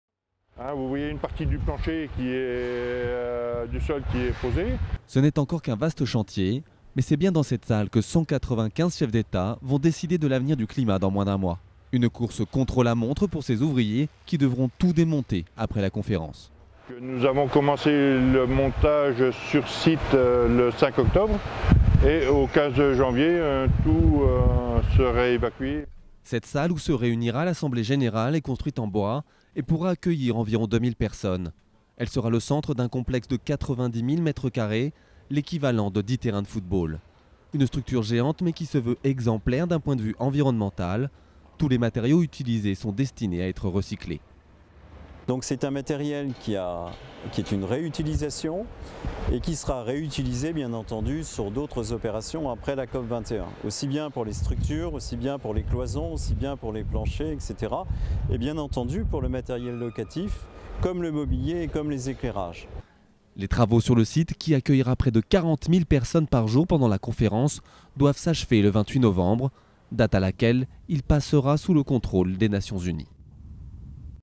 COP 21: visite des travaux au Bourget un mois avant le lancement
Dans un mois, la grande conférence mondiale sur le climat s’ouvrira à Paris pour trouver un accord mondial sur le réchauffement climatique. Le site du Bourget qui accueillera les 195 chefs d’État est toujours en travaux. Près de 90.000 m2 seront aménagés pour accueillir les 40.000 visiteurs quotidiens. Le chantier doit s’achever le 28 novembre avant de passer sous le contrôle des Nations-Unies.